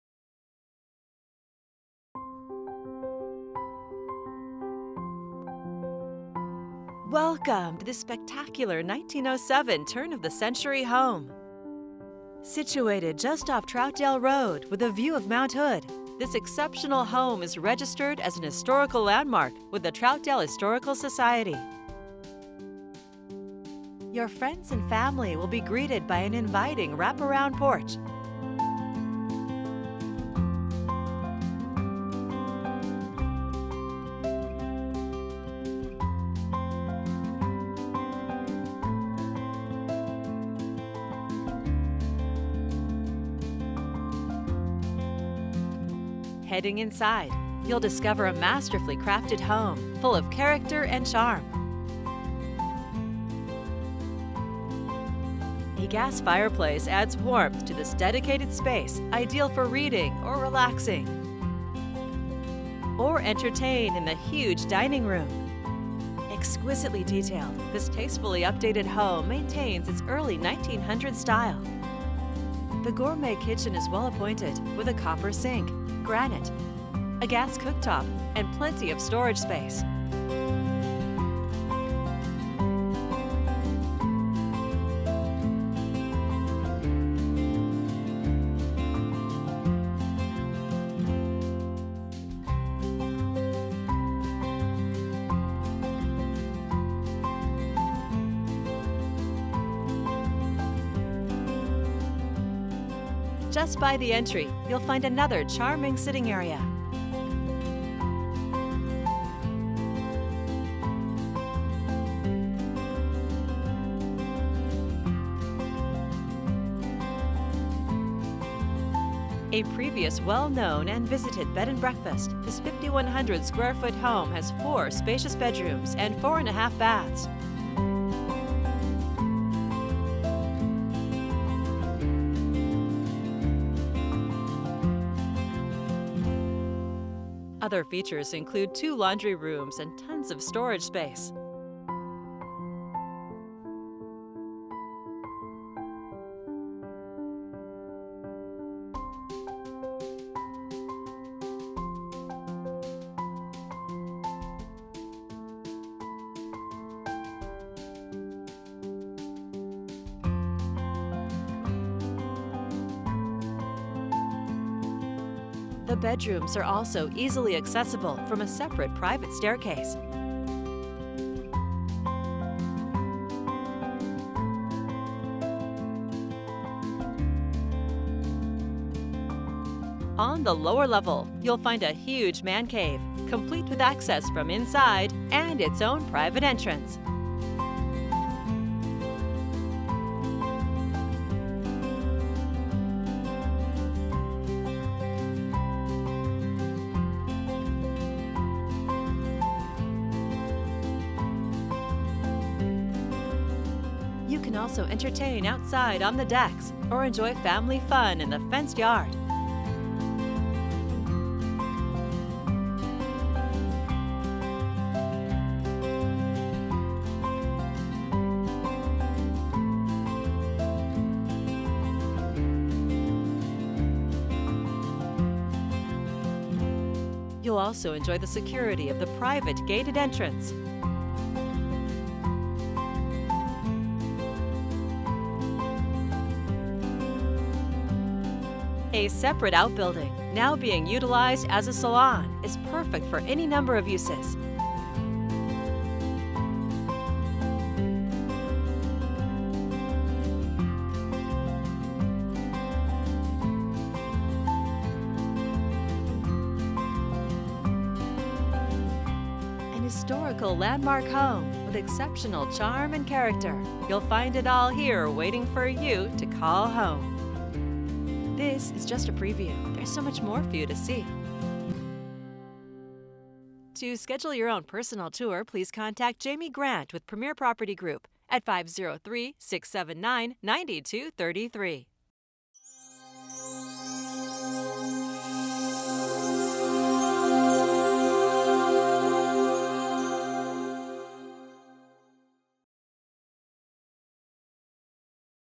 Historical Home and Former Bed and Breakfast for sale ~ Video of 2611 S. Troutdale Rd.
This 1907 Turn of the Century home sits up off Troutdale Rd with a view of Mt Hood. Registered as a Historical Landmark with the Troutdale Historical Society, this 4 bedroom, 4.1 bath home is full of character and charm! Tasteful upgrades that keep the early 1900’s feel. A previous well known and visited Bed and Breakfast. 5100 sq. ft. of living space, gourmet kitchen, and lots of space for entertaining and guests! Contact Jaime Grant with Premiere Property Group for more information or your personal tour.